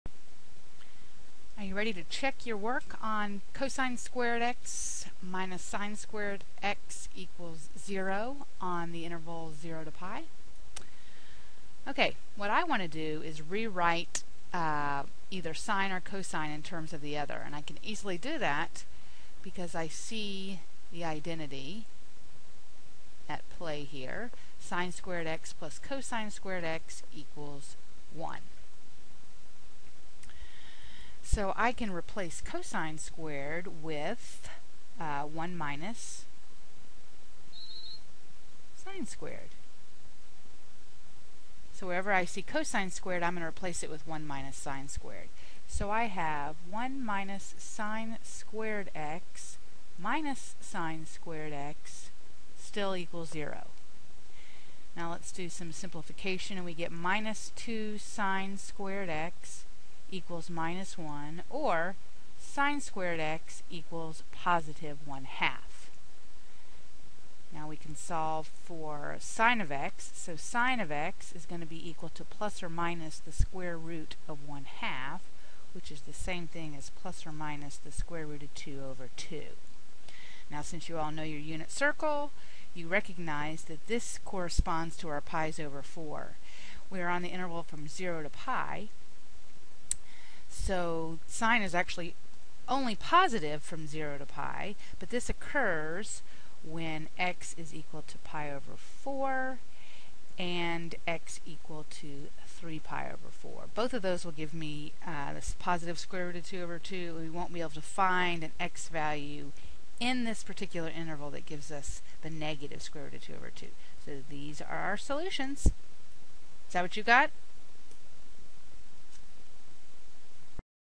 Solving trig equation
uses pythagorean identity, solutions in (0,pi)